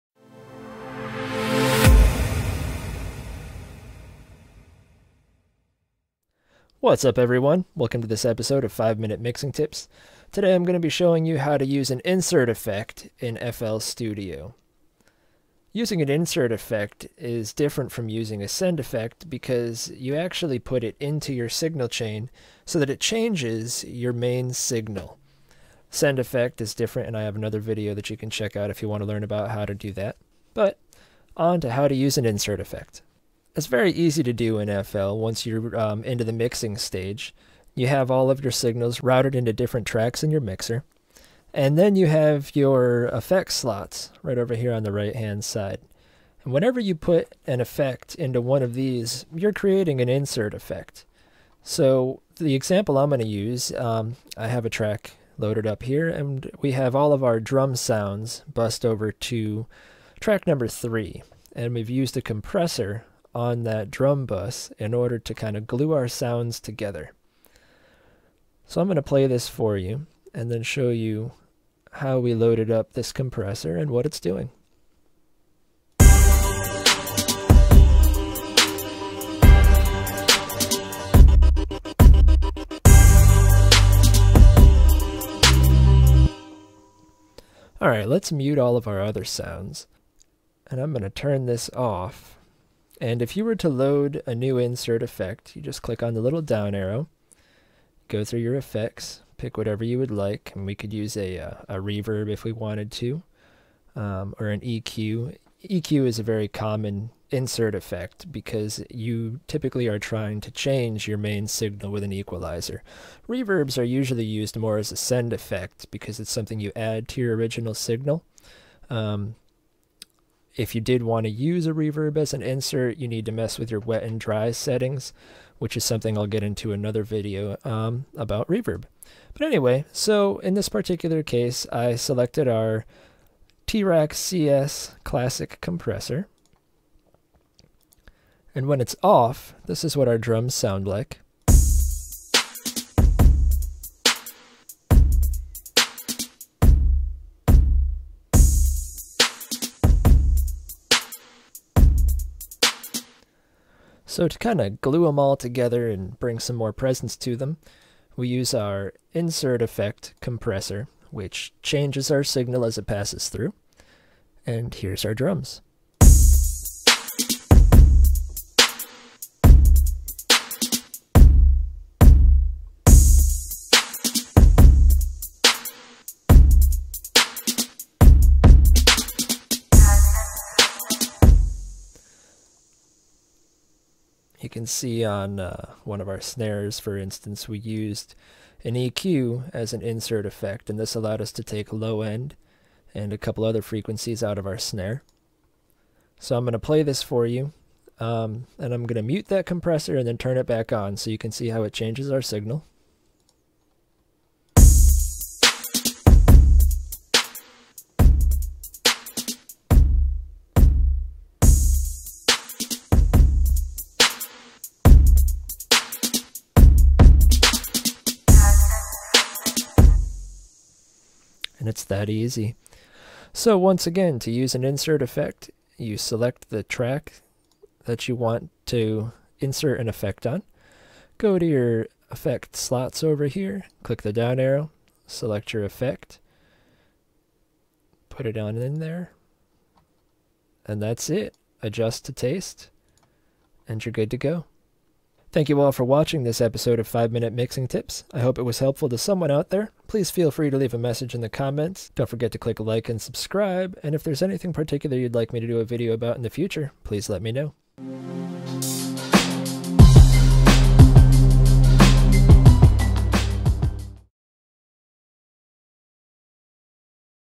How to Use Insert Effects in FL Studio - Insert Compression Flanger and More - 5 Minute Mixing Tips
Mix like the pros with insert effects in FL Studio. Learn how to insert compression, EQ, Phasers, Flangers and more directly into your audio signal.